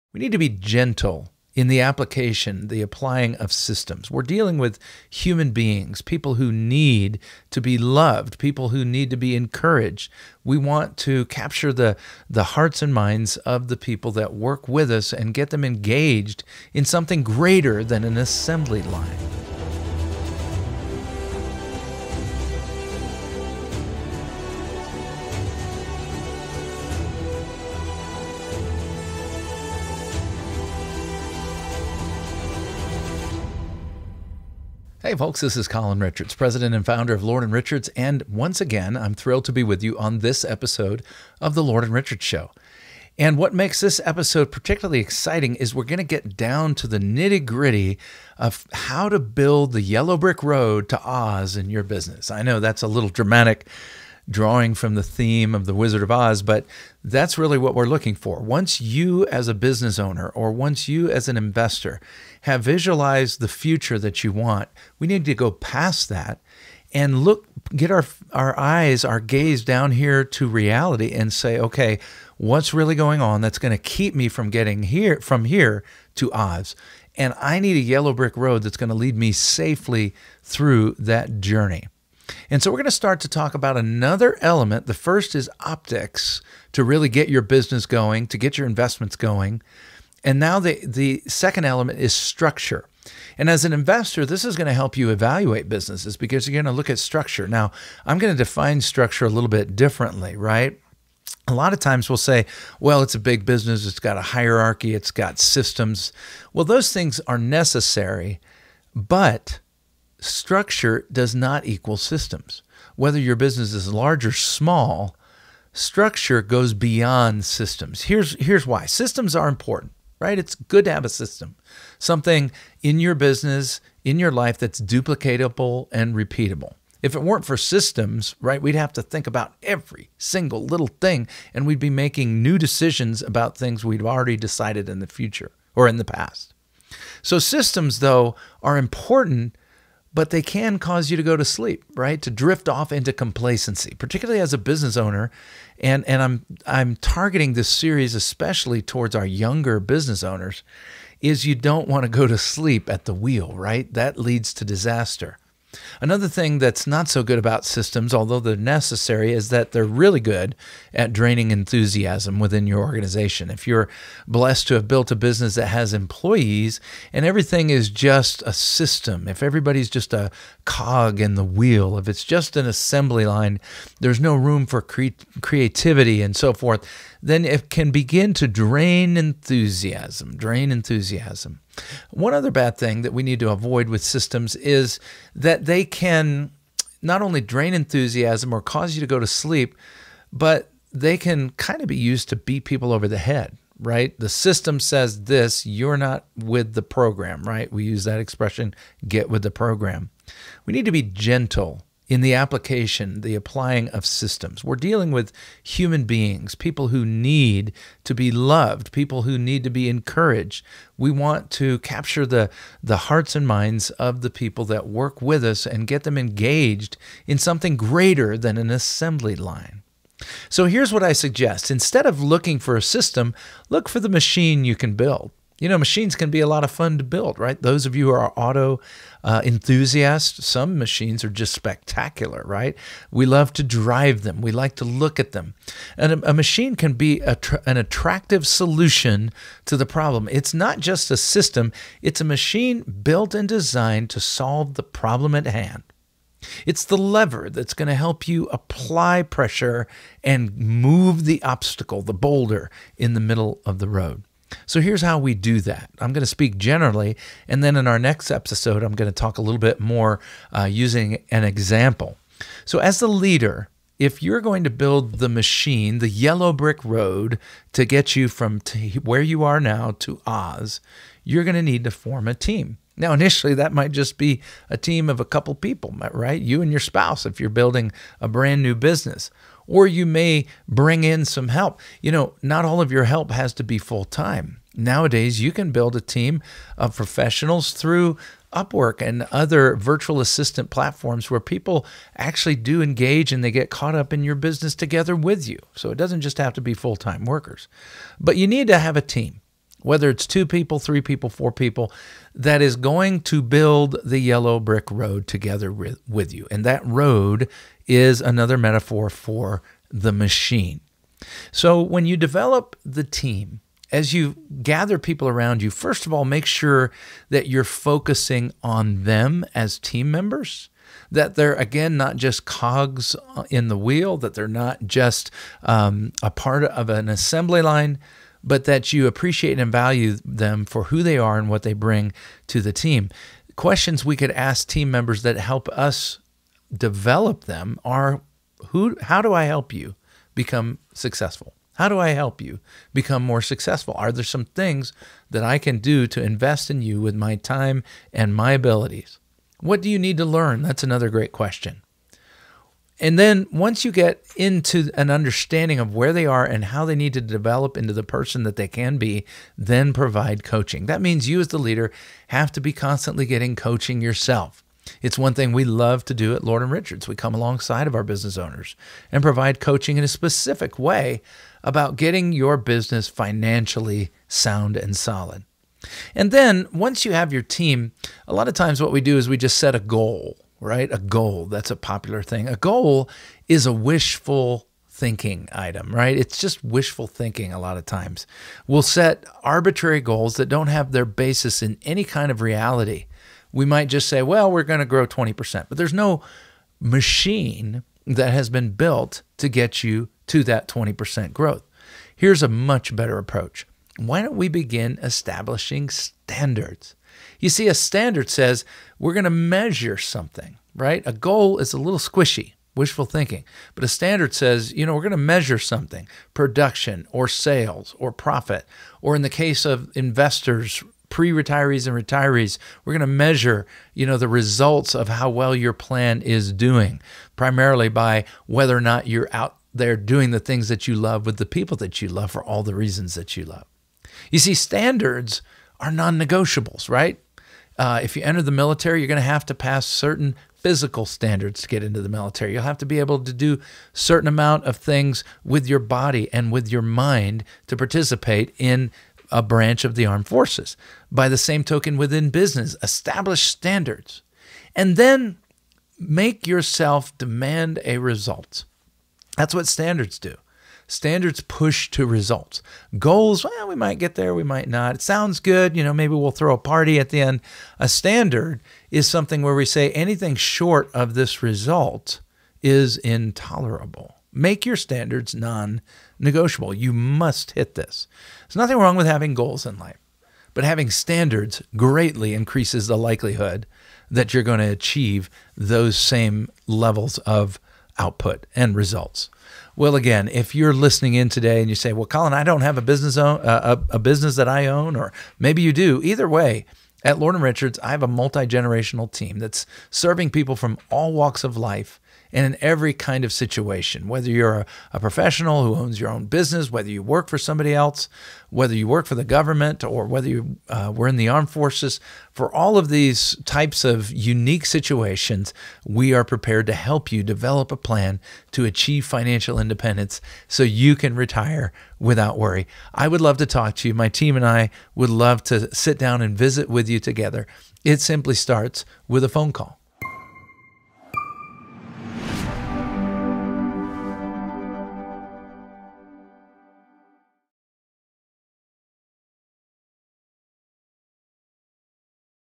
Building the Yellow Brick Road to Success
Join Colin Richards, president and founder of Lauren Richards, in this captivating episode of the Lord and Richard Show. In today's discussion, we're diving into the essential elements of building your path to success, inspired by the classic tale of the Wizard of Oz.

Moving beyond mere visualization, we'll explore the importance of structure in your business or investment journey. Discover why structure goes beyond systems and how it can help you avoid complacency, maintain enthusiasm, and foster creativity within your organization.

Colin redefines the concept of structure and introduces the idea of building a machine that solves problems, rather than relying solely on systems. Learn the significance of forming a dedicated team to help construct your "yellow brick road" and the importance of valuing each team member's unique contributions.

Colin shares insightful questions to ask yourself and your team to facilitate growth and development, emphasizing the need for coaching and continuous improvement. Additionally, discover the power of establishing standards as non-negotiable benchmarks for success, pushing you toward results that are truly intolerable to fall short of.

Whether you're a business owner or an investor, this episode offers valuable insights that can help you navigate your journey toward financial independence.

If you watch the video and have questions you’d like to ask personally, here’s a link to set up a quick 15-minute call with Colin’s team.